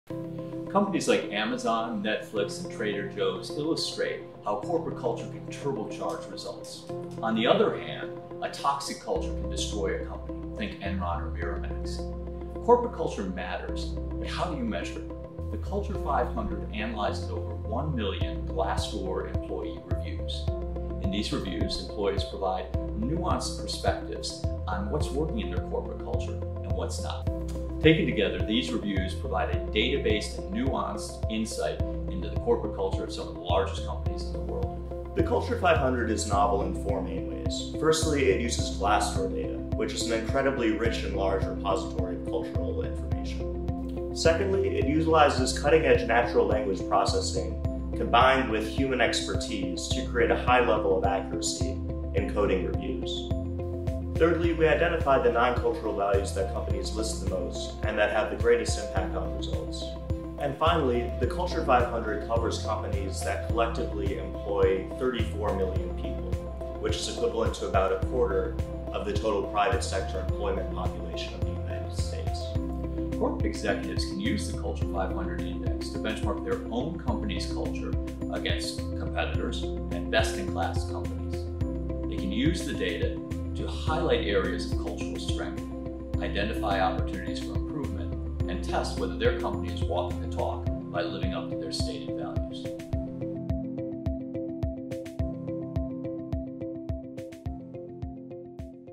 Introducing the MIT SMR/Glassdoor Culture 500
The MIT SMR/Glassdoor Culture 500 brings together MIT expertise and cutting-edge AI to compare and rank the companies leading the U.S. economy using a data set of more than one million Glassdoor reviews.

Featuring: The Culture 500 Research Team — Donald Sull and Charlie Sull, cofounders of CultureX